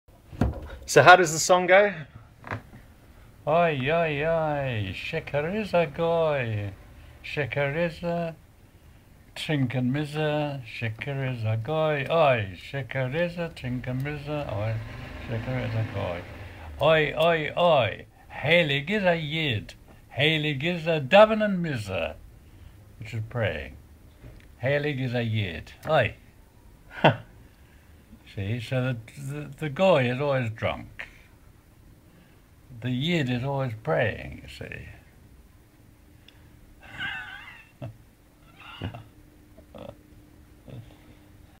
Emanuel Litvinoff recites old Yiddish ghetto song
No opinions are actually expressed here - he's just reciting the song!